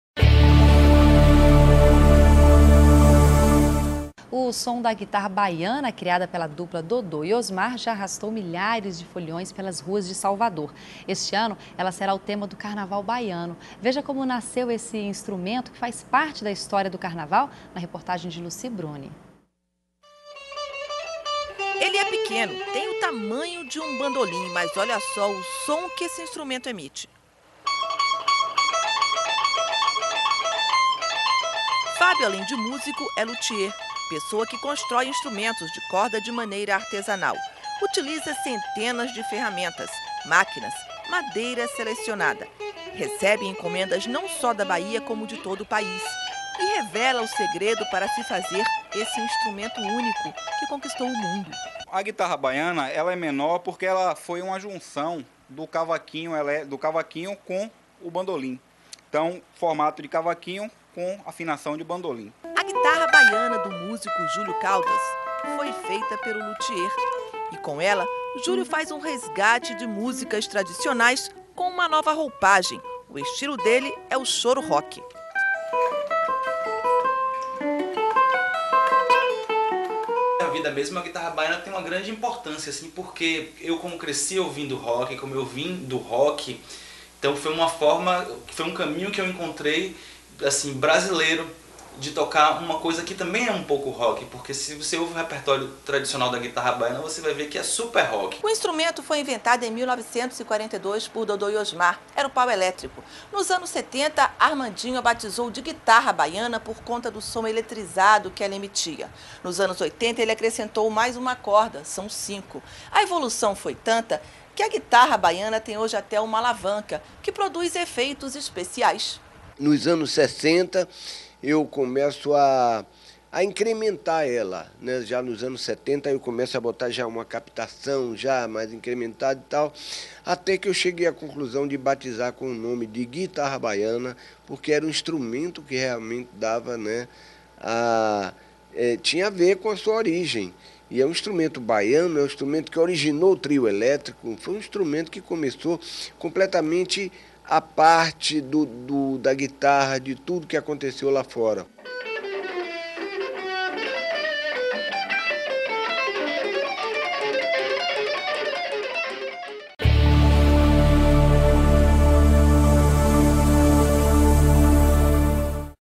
Guitarra baiana é tema de carnaval de Salvador
O som da guitarra baiana criada pela dupla Dodô e Osmar já arrastou milhares de foliões pelas ruas de Salvador. Este ano, ela será o tema do carnaval baiano.